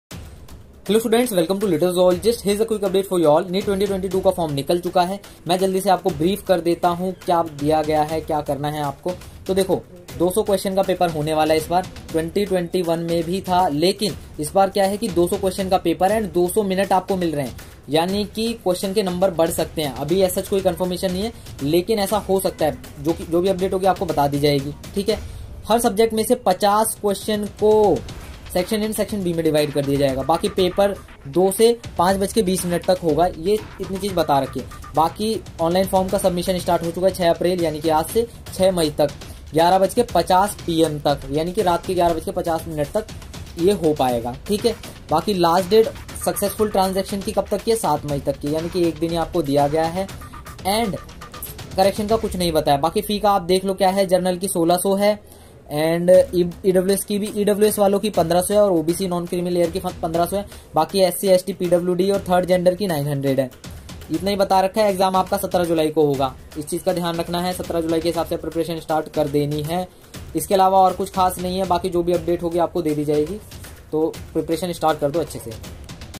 NEET 2022 latest update | exam on 17 july 2022 | official confirmation
RELEVANT UPLOADS

Tags (ignore) :-

neet 2022 application,
neet 2022 application form date by nta,
when will be neet 2022 be conducted